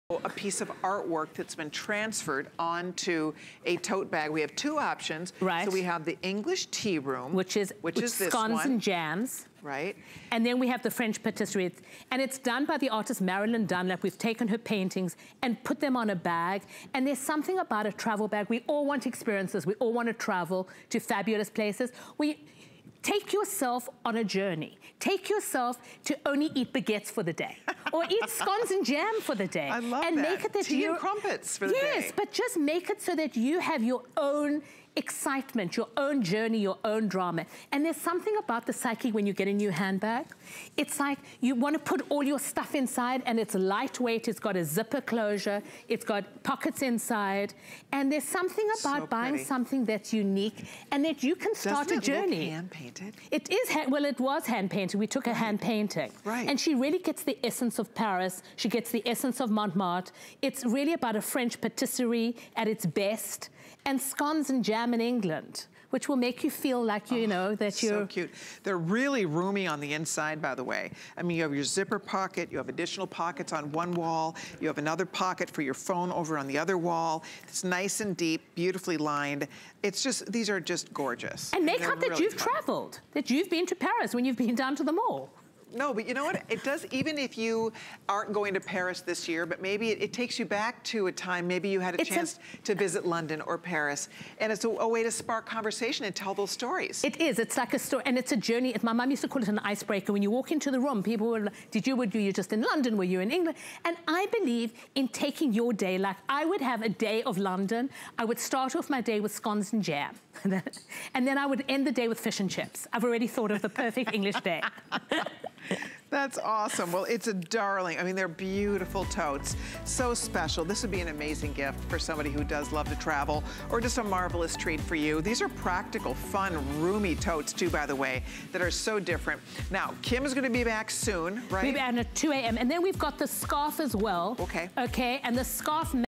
Clever Carriage Travel Shopper
Kim Isaacsohn, Head Coachman of Clever Carriage Company, absolutely loves scenes of French Patisseries and English Tea Rooms. These gorgeous shoppers have the scenes printed onto manmade material on both sides of the bag. It's a perfect lightweight travel tote to take with you on your journey fit all of your essentials and feel as if you are in France or England, no matter where you are!